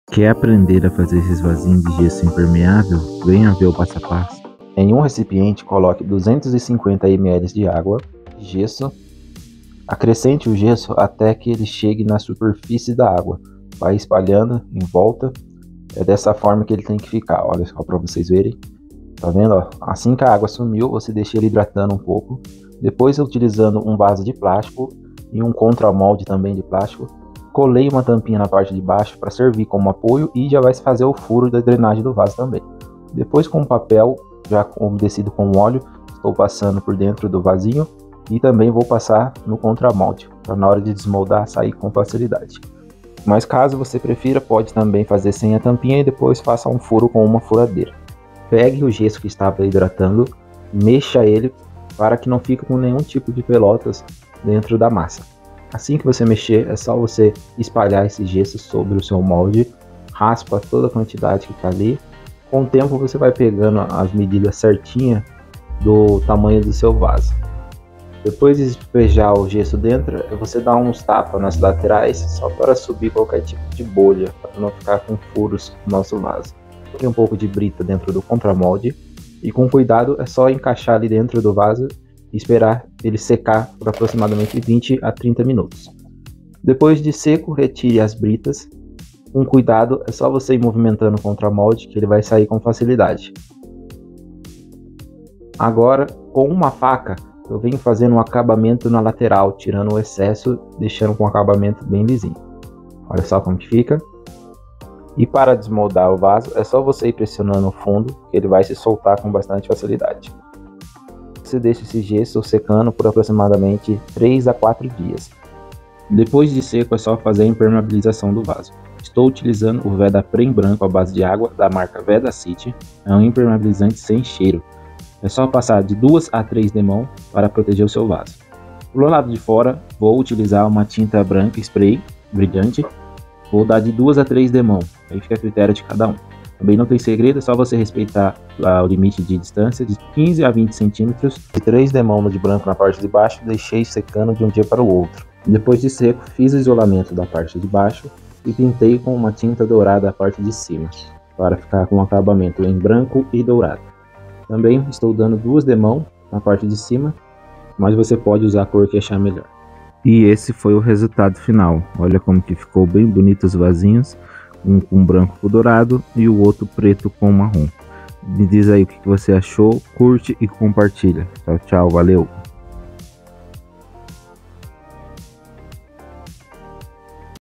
Veja o jeito mais fácil de fazer vasos decorativos para sua casa!
Nesse vídeo vou  mostrar como é fácil fazer vasos decorativos para sua casa, projeto DIY super fácil.